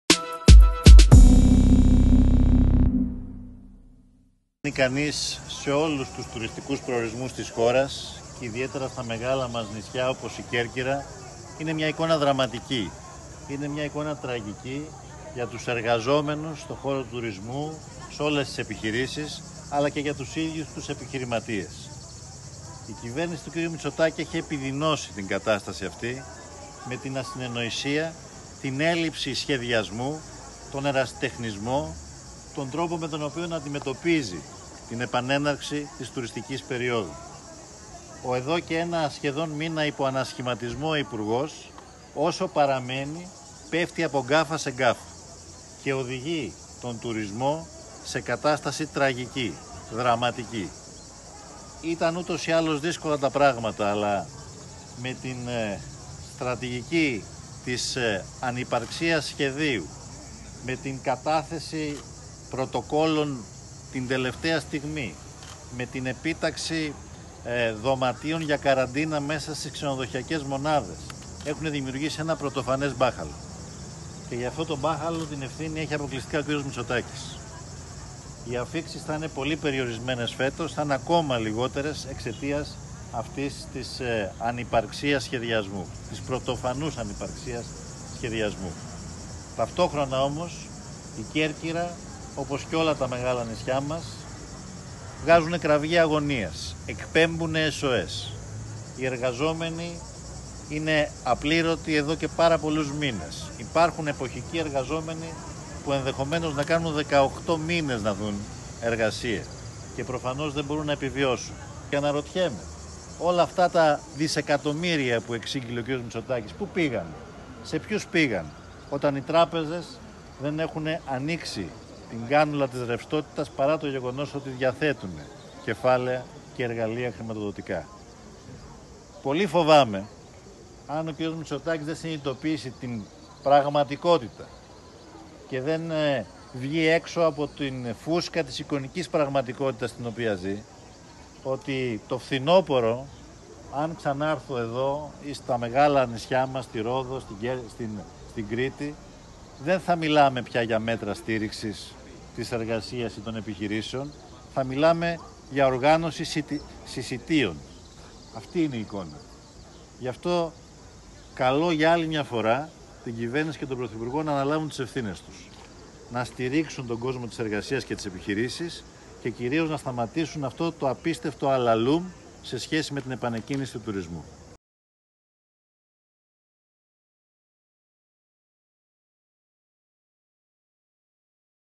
Αλ. Τσίπρας από Κέρκυρα: Αποκλειστική ευθύνη Μητσοτάκη το μπάχαλο στον τουρισμό
Δριμεία κριτική στην κυβέρνηση και προσωπικά στον πρωθυπουργό για «ανυπαρξία σχεδίου, ασυνεννοησία, ερασιτεχνισμό», με αποτέλεσμα ένα «πρωτοφανές μπάχαλο» στην επανεκκίνηση του τουρισμού, άσκησε ο πρόεδρος του ΣΥΡΙΖΑ, Αλέξης Τσίπρας, από την Κέρκυρα.

   Ο αρχηγός της αξιωματικής αντιπολίτευσης μίλησε για «δραματική και τραγική» εικόνα σε όλα τα μεγάλα νησιά, για εργαζόμενους και επιχειρήσεις, επιρρίπτοντας στην κυβέρνηση την ευθύνη για την επιδείνωση της κατάστασης. Κάλεσε την κυβέρνηση και τον κ. Μητσοτάκη «να αναλάβουν τις ευθύνες τους, να στηρίξουν τον κόσμο της εργασίας και τις επιχειρήσεις και κυρίως να σταματήσουν αυτό το απίστευτο αλαλούμ σε σχέση με την επανεκκίνηση του τουρισμού». Σε διαφορετική περίπτωση, τόνισε, το φθινόπωρο δεν θα μιλάμε για στήριξη της εργασίας και των επιχειρήσεων, αλλά για οργάνωση συσσιτίων.

   «Η εικόνα που προσλαμβάνει κανείς σε όλους τους τουριστικούς προορισμούς της χώρας και ιδιαίτερα στα μεγάλα νησιά όπως η Κέρκυρα, είναι μια εικόνα δραματική, εικόνα τραγική για τους εργαζόμενους στο χώρο του τουρισμού, σε όλες τις επιχειρήσεις, αλλά και στου ίδιους τους επιχειρηματίες», δήλωσε μετά από την περιοδεία του στο Κέντρο της πόλης του νησιού. Είπε ότι «η κυβέρνηση Μητσοτάκη έχει επιδεινώσει την κατάσταση αυτή με την ασυνεννοησία, με την έλλειψη σχεδιασμού, τον ερασιτεχνισμό, τον τρόπο με τον οποίο αντιμετωπίζει την επανέναρξη της τουριστικής περιόδου», προσθέτοντας: «Ο εδώ και έναν μήνα υπό ανασχηματισμό υπουργός όσο παραμένει πέφτει από γκάφα σε γκάφα και οδηγεί τον τουρισμό σε κατάσταση τραγική, δραματική». «Ήταν ούτως ή άλλως δύσκολα τα πράγματα αλλά με την στρατηγική της ανυπαρξίας σχεδίου, με την κατάθεση πρωτοκόλλων την τελευταία στιγμή, με την επίταξη δωματίων για καραντίνα μέσα στις ξενοδοχειακές μονάδες, έχουν δημιουργήσει ένα πρωτοφανές μπάχαλο και για το μπάχαλο αυτό την ευθύνη έχει αποκλειστικά ο κ. Μητσοτάκης», τόνισε ο πρόεδρος του ΣΥΡΖΑ. Υποστήριξε ότι Οι αφίξεις θα είναι πολύ περιορισμένες φέτος και θα είναι ακόμα λιγότερες εξαιτίας της «πρωτοφανούς ανυπαρξίας σχεδιασμού».